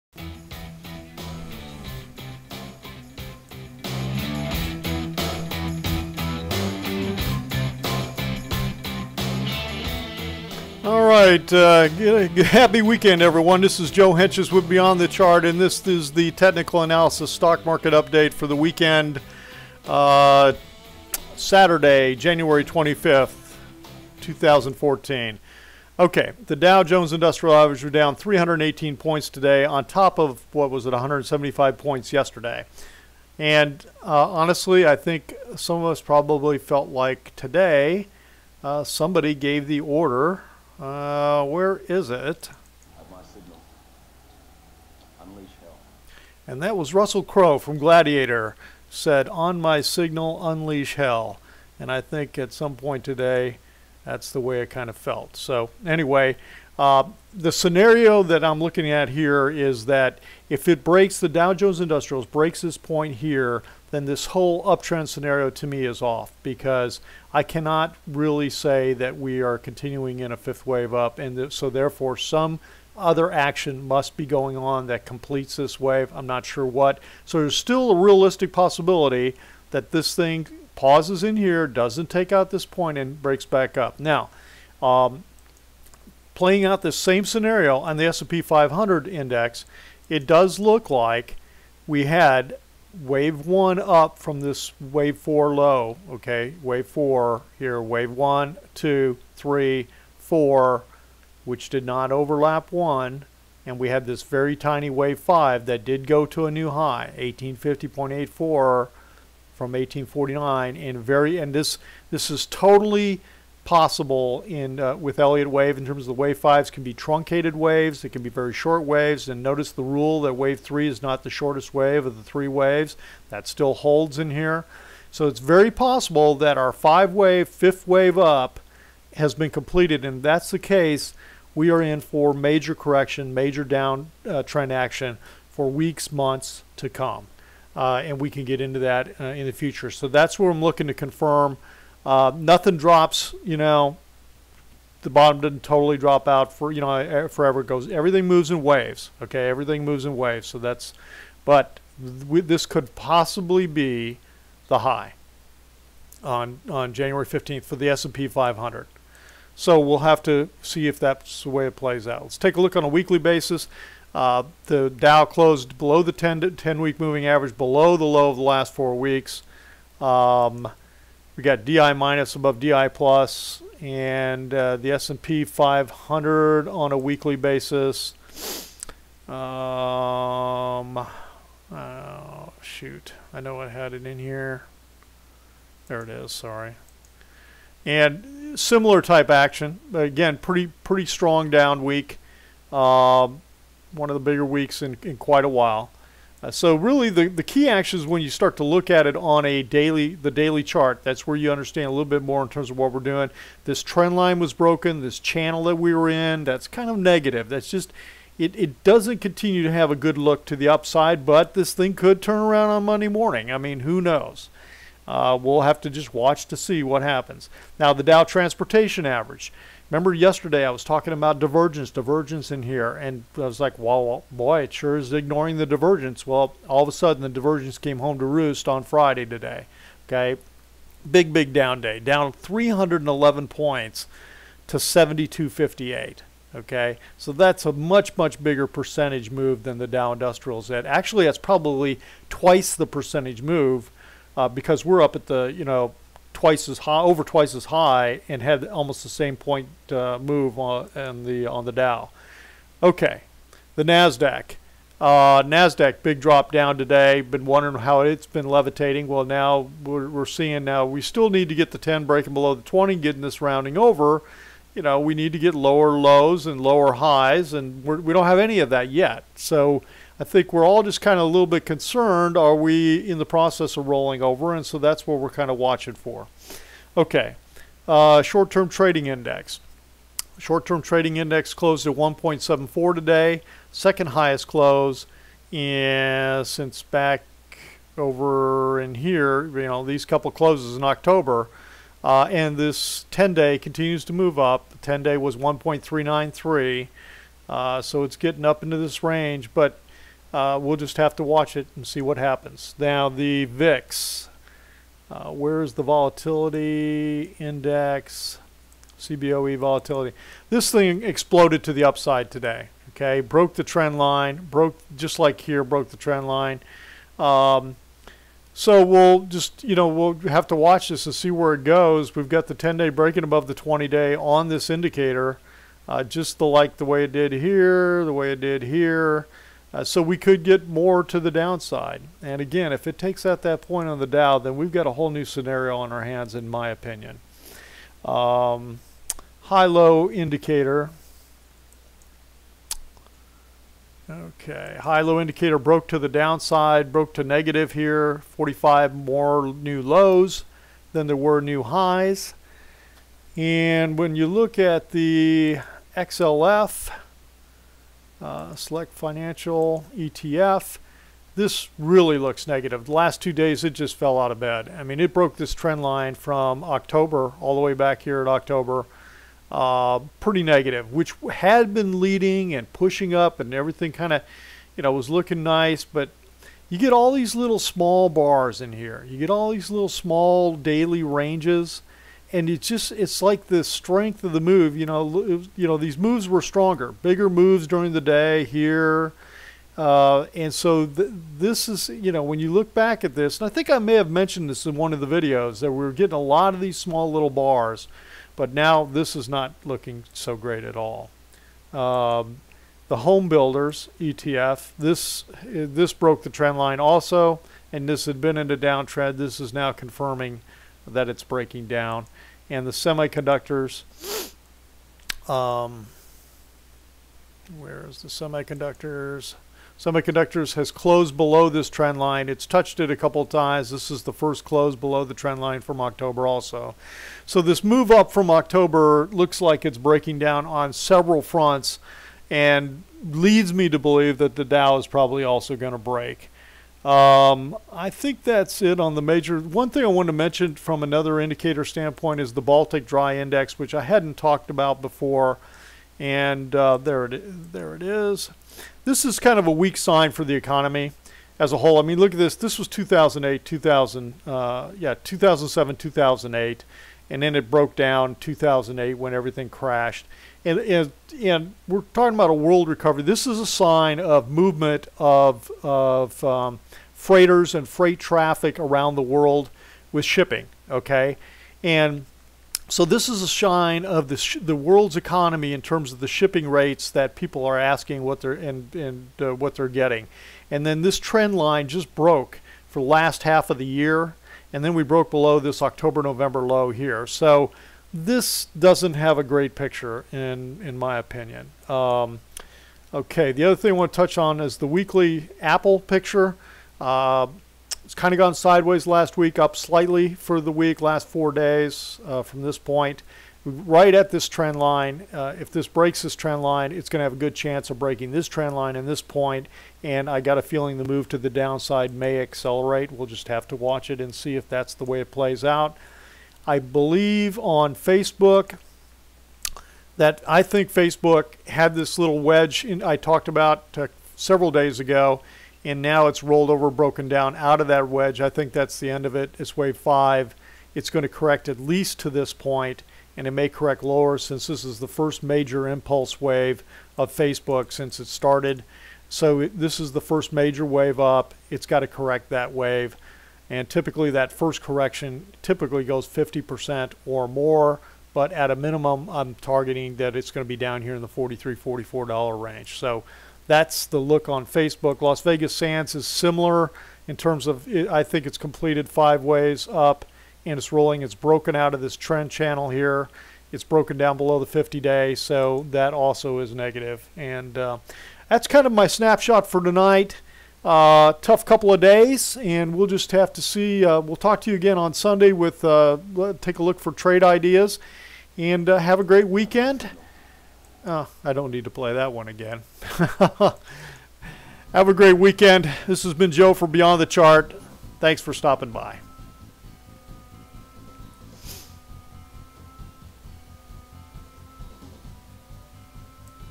Beginning or end? | Weekend Stock Market Update | 01/25/14 | Beyond the Chart
In this technical analysis stock market update video we review the market action and try to assess what is happening.  We look at the Dow Industrials and the S&P 500 wave structure for clues.  The Dow Transports got hit twice as bad as the Industrials but they also had not sold off on Thursday.  is this the beginning of a much bigger correction or the end of a short one?
We also look at our regular indicators and for the first time look at the Baltic Dry Index to see what it might be telling us.
We take our reular weekly view of Apple (AAPL) and also check Facebook (FB) and Las Vegas Sands (LVS)
In my post on my website I discuss more about the VIX.
Index
00:00  Indices
04:39  Indicators
05:59  XLF, XHB, SMH
07:27  Baltic Dry Index
08:14  AAPL, FB, LVS